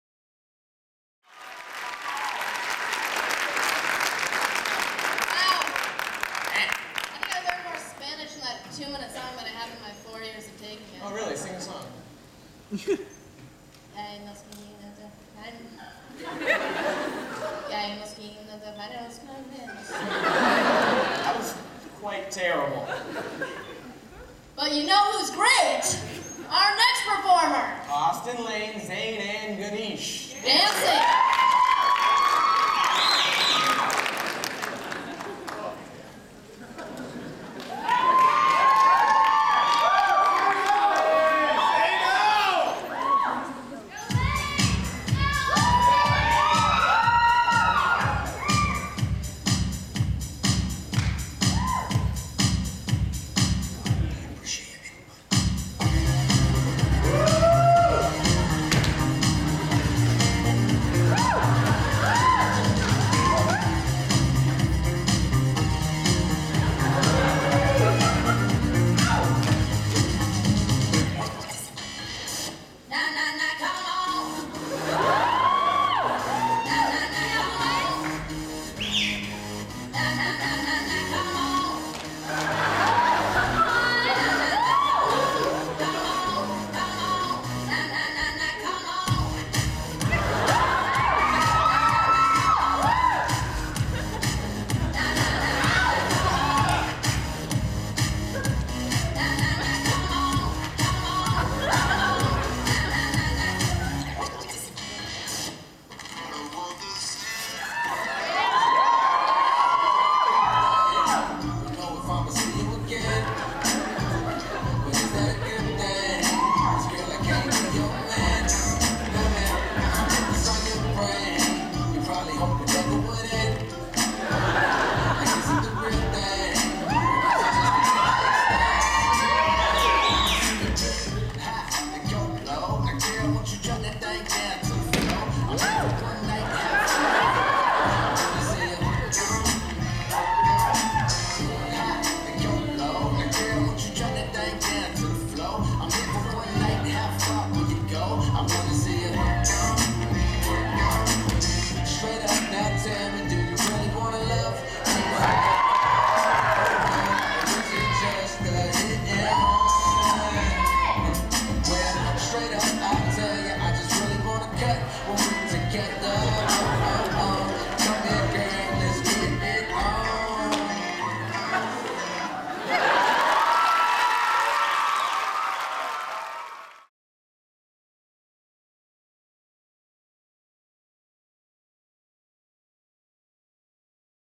Mr. Mustang: Dance Party 2007, No Regretzz
Ganesh Shivaram, Austin Wride, Zane Gray & Lane Luttrell dance to a mashup of songs.